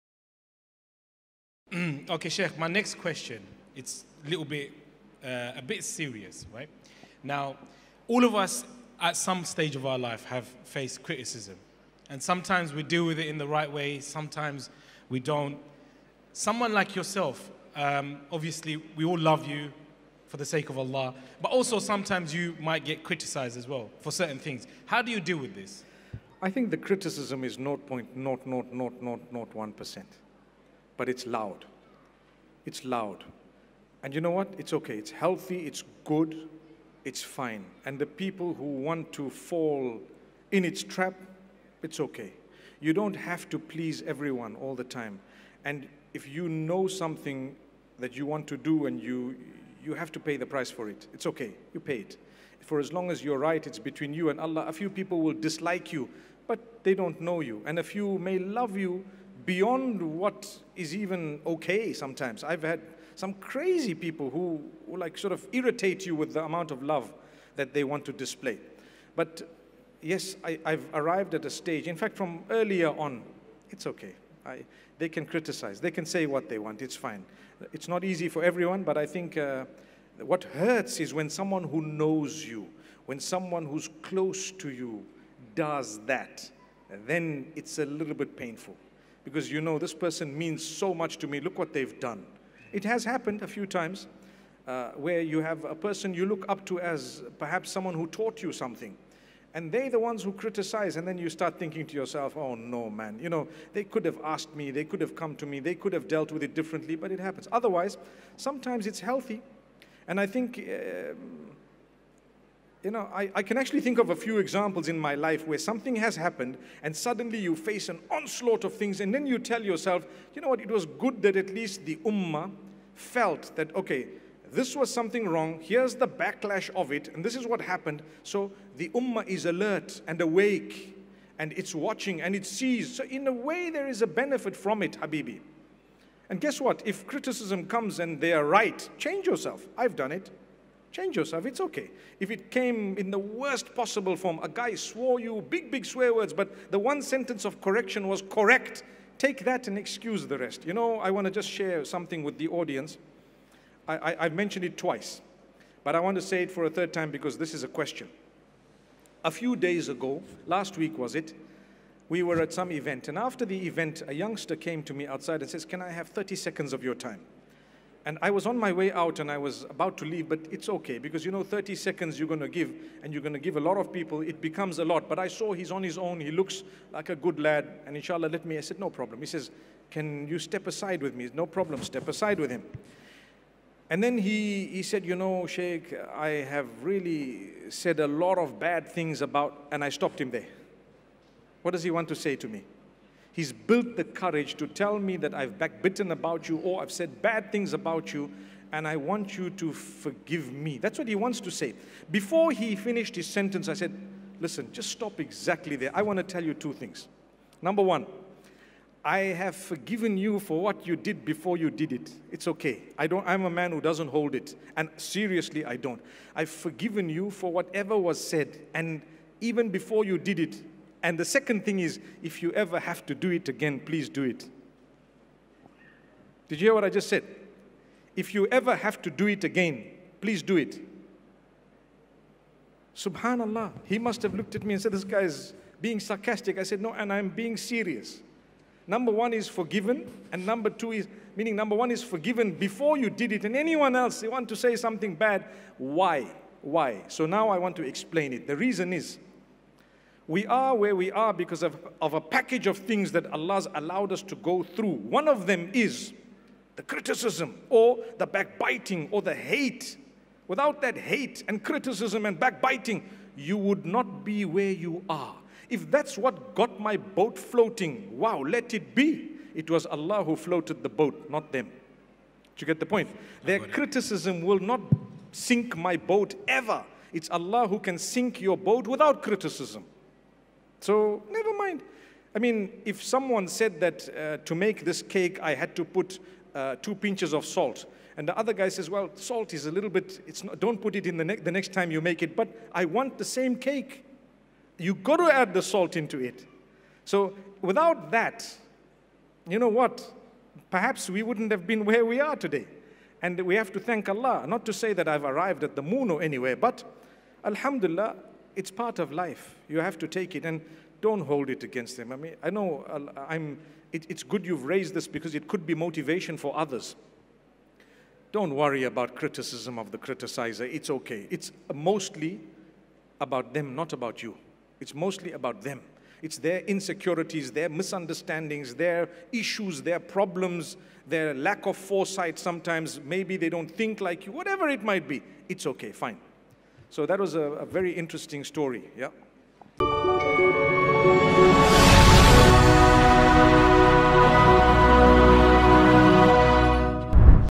How do you deal with criticism? How to try to remain calm and composed - Mufti Ismail Menk
In this vedio, Mufti Menk is  speech of  specifically criticism , He gave the information  about  on this topic, he provide the general advice on how to handle criticism and stay composed. how to  try to remain calm and composed.
Mufti Ismael Menk, a prominent Islamic scholar, often emphasizes the importance of remaining calm and composed when faced with criticism.